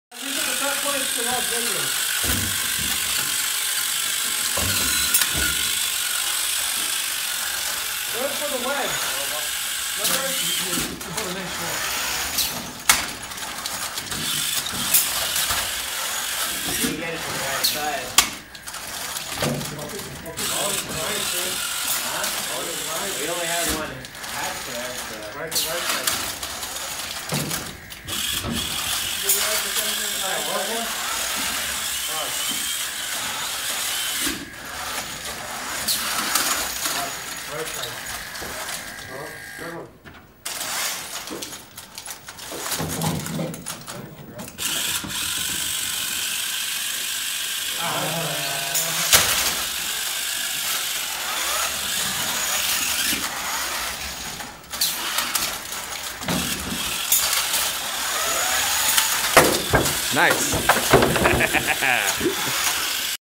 Precision driving
Test driving our 2019 FRC robot.